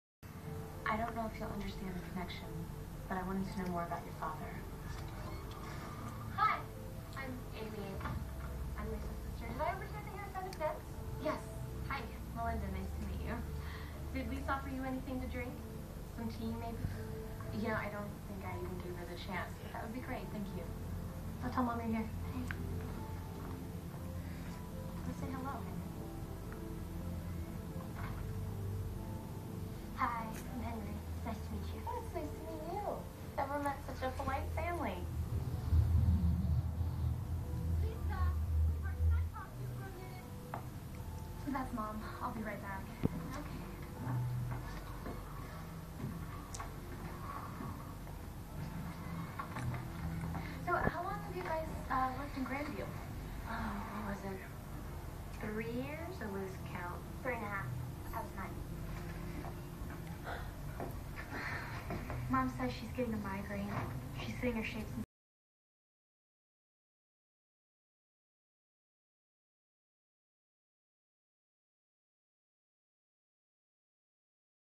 SLOW LORIS___Rocky being spoiled
Spoil baby Slow Loris Rocky Starlight.. Very cute one and loved to be tickled..